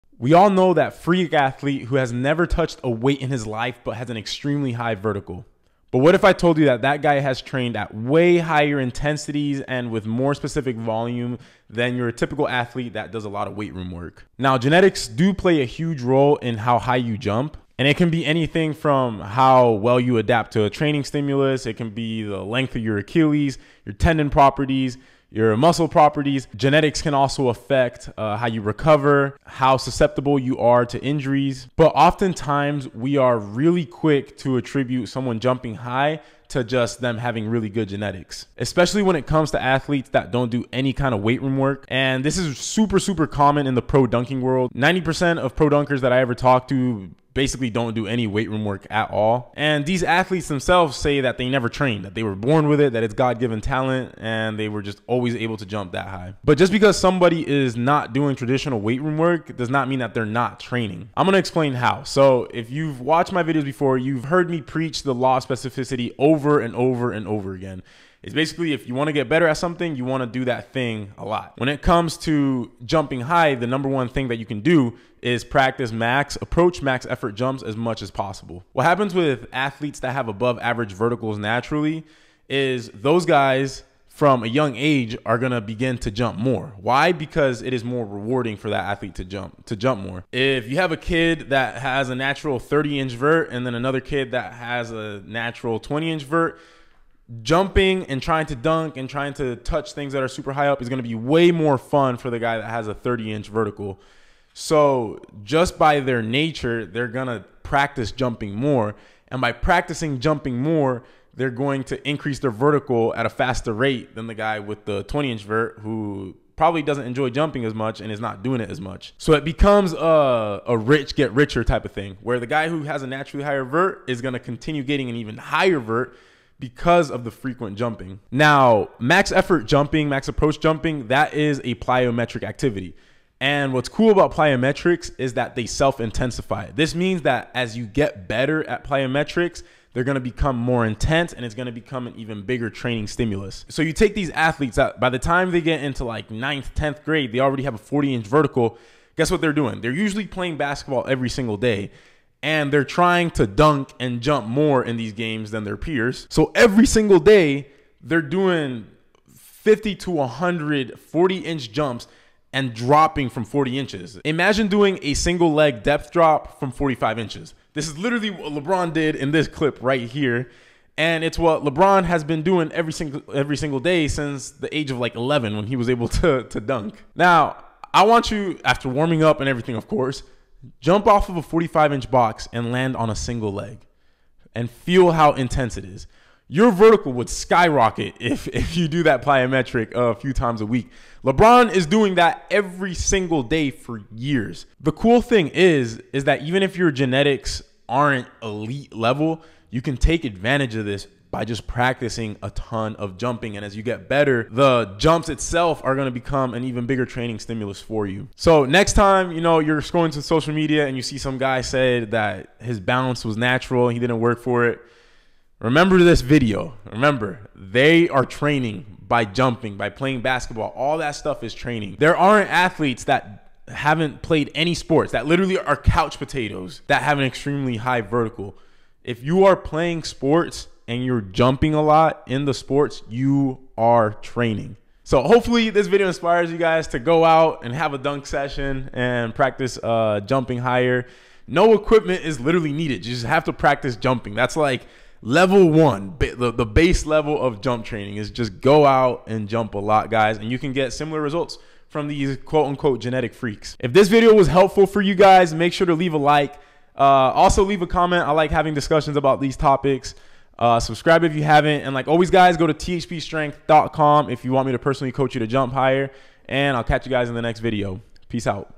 How Do Genetic Freaks Jump So High?
In today's video I explain the common misconception that athletes who naturally jump high don't train at all.